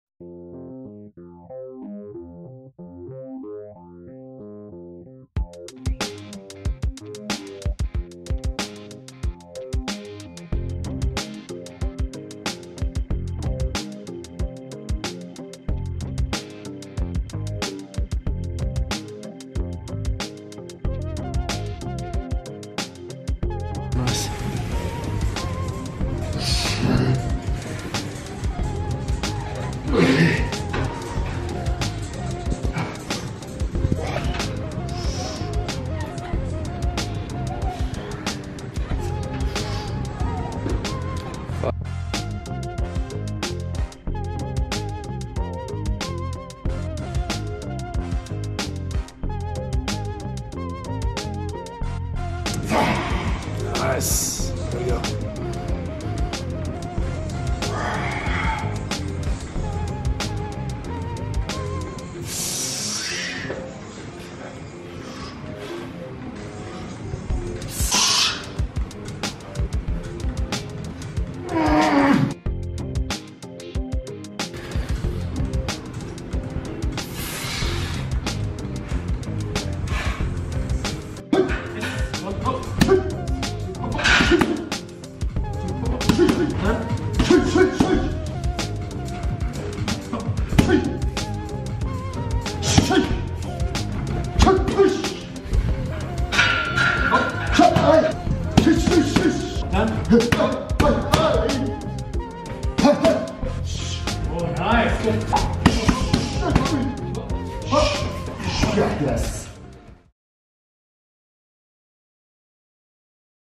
Ultimate Recovery & Striking Workout with NJPW's HENARE
Are you ready to witness the secrets behind the incredible athleticism of New Japan Pro Wrestling's (NJPW) Henare? Join us in this exclusive workout video as we take you behind the scenes of a powerful recovery and striking session, designed to prepare Henare for his intense competition in the G1 Climax!

Don't miss this opportunity to train like a pro with Henare himself. Level up your fitness, amplify your striking power, and boost your recovery game. Get ready to dominate the mat with the fierce strength and determination of an NJPW superstar!

More is coming soon